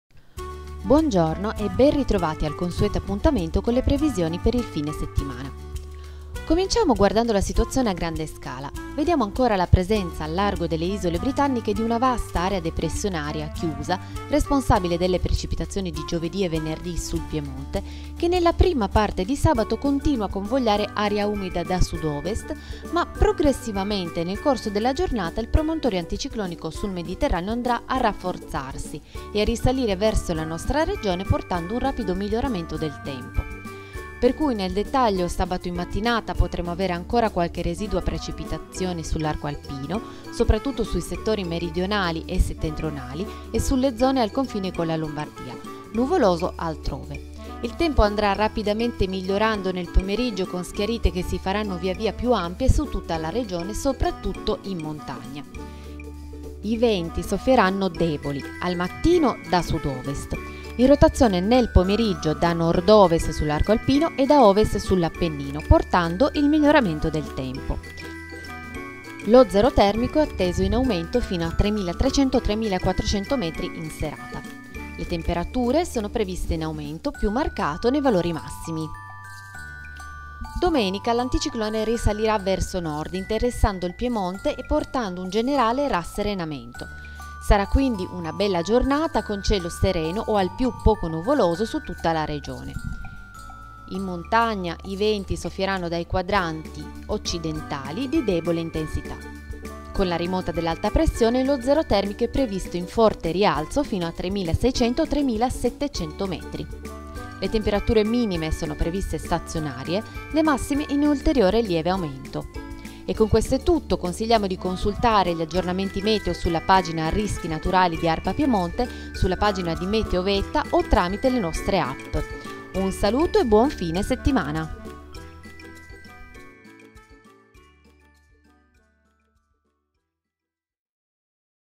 Previsioni meteorologiche per il fine settimana del 15-16 ottobre 2016 in Piemonte - Meteovetta
Previsioni Meteo Meteovetta sul Piemonte per il weekend  15 - 16 Ottobre 2016.
Elaborazione ARPA Piemonte